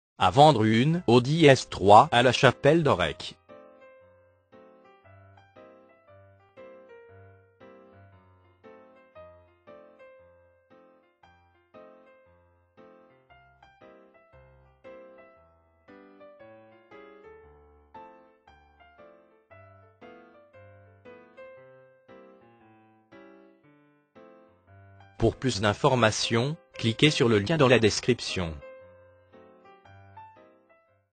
Achat Vente une Audi S3 La Chapelle d Aurec
A vendre à  La Chapelle d Aurec  Occasion automobile Audi S3 de 32000 Km.
automobile occasion Autoenvideo : Achat Vente une Audi S3  La Chapelle d Aurec  Haute-Loire

Création du diaporama vidéo par API pour professionnel de l automobile